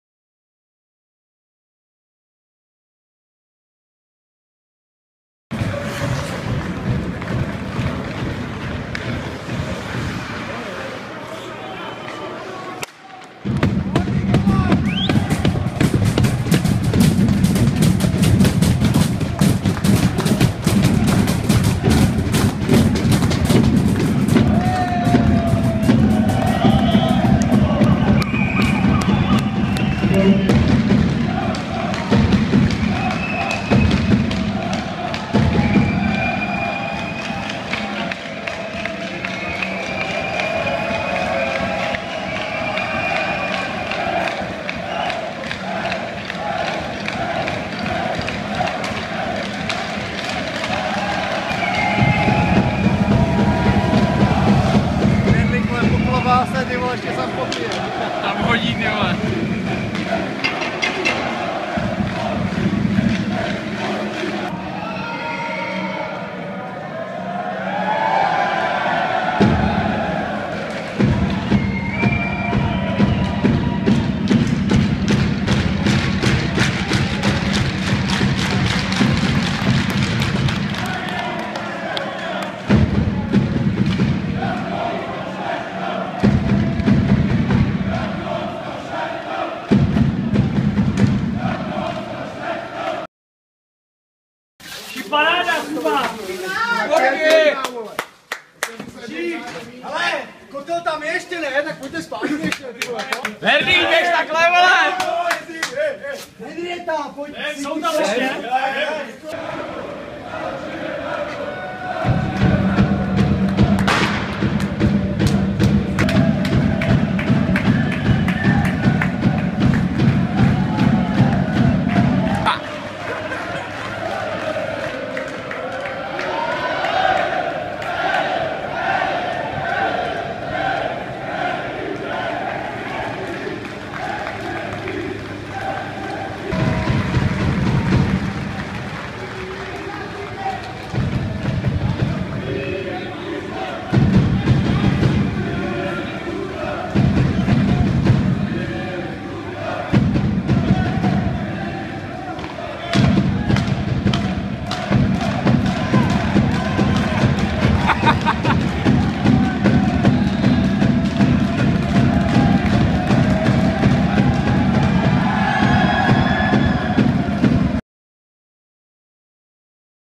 "Knedlík po klobáse" aneb takhle se fandí v Náchodě!
Děkujeme fanouškům za skvělou podporu během utkání proti HC BAK Trutnov! Byli jste skvělí!